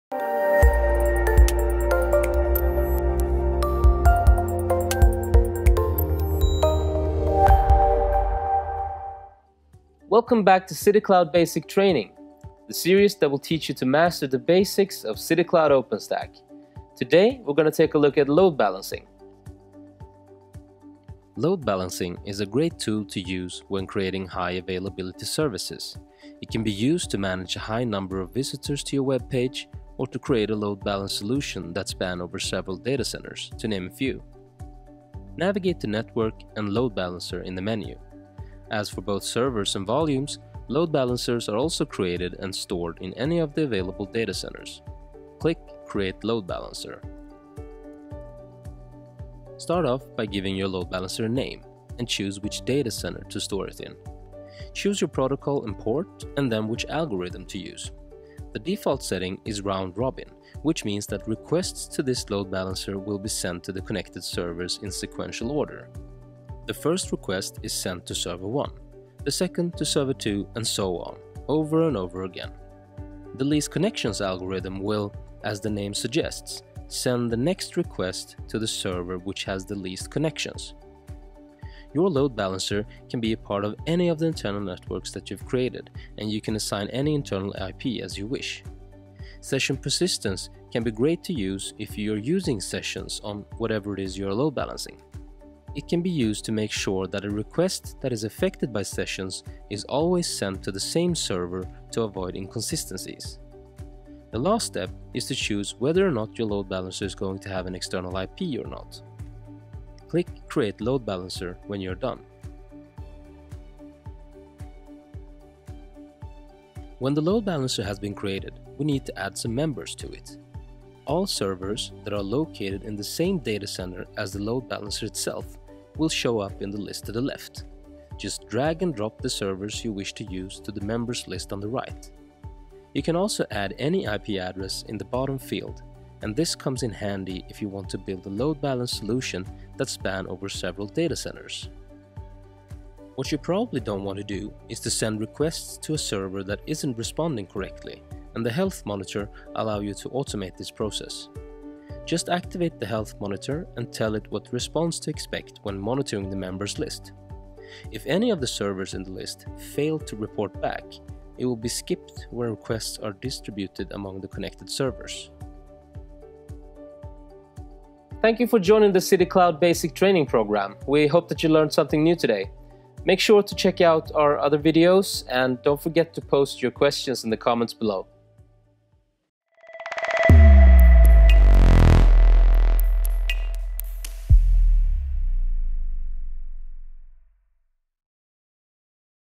Load Balancing
- City Cloud OpenStack Basic Training

Learn how to create and manage Load Balancers in City Cloud OpenStack.